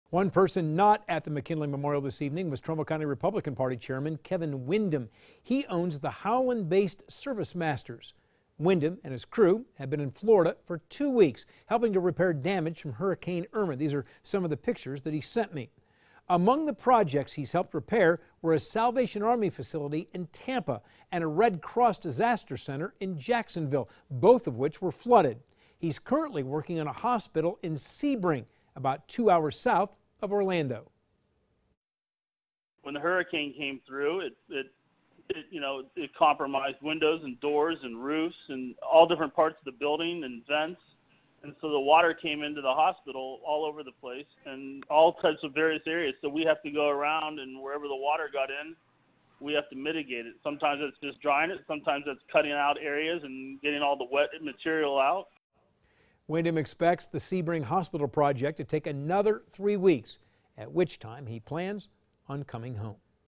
Trumbull Republican chair in Florida for hurricane repair work
Kevin Wyndham and his crew have been in Florida for two weeks helping to repair damage from Hurricane Irma.